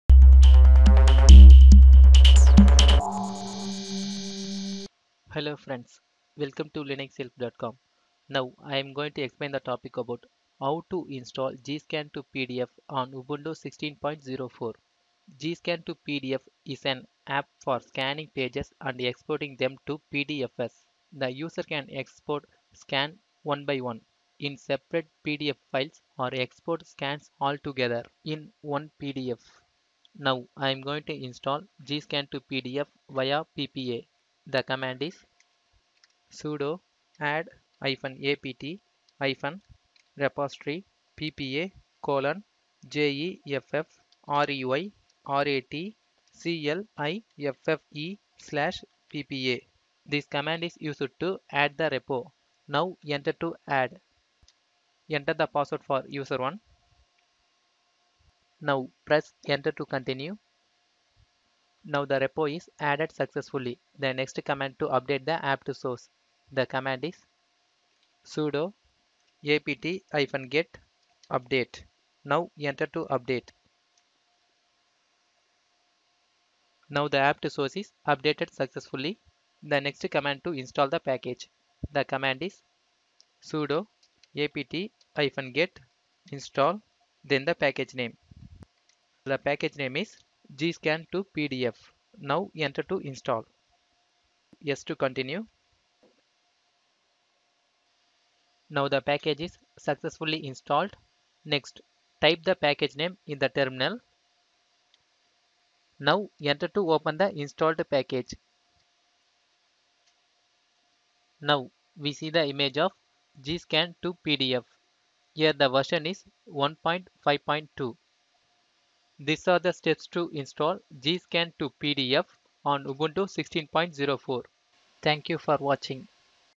How to install GScan2PDF in Ubuntu
This video explains how to install GScan2PDF - It is an open source app used to scan and export the documents into PDF format. GScan2PDF is very easy to use and supports many Linux distro.